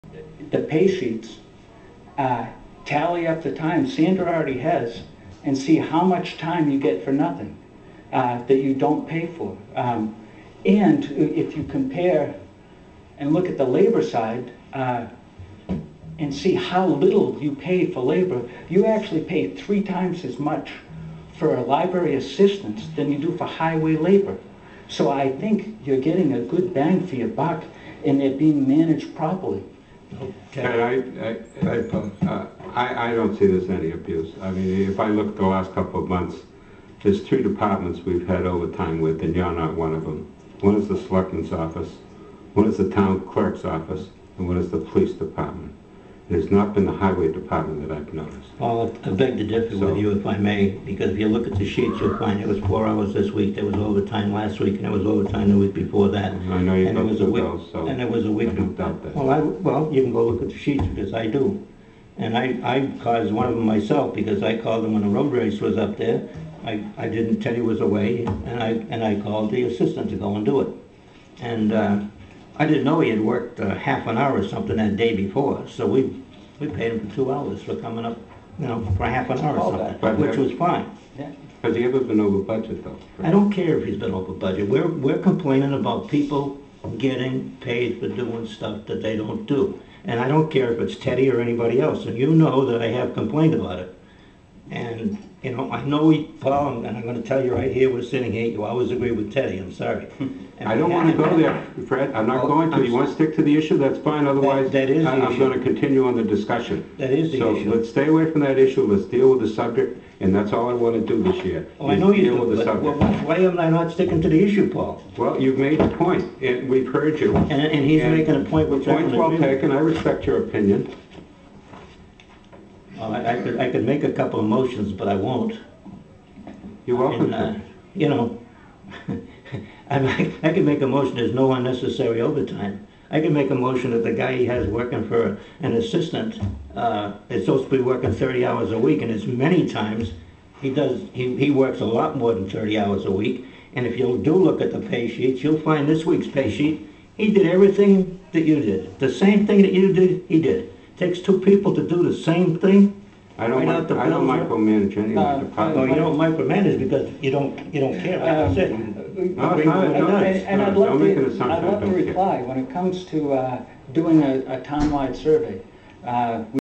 Selectman meeting 4/21/08 Fred Childs picking on Ted
Wow does Fred Childs have it out for Ted Stewart.  Looking good Fred Childish!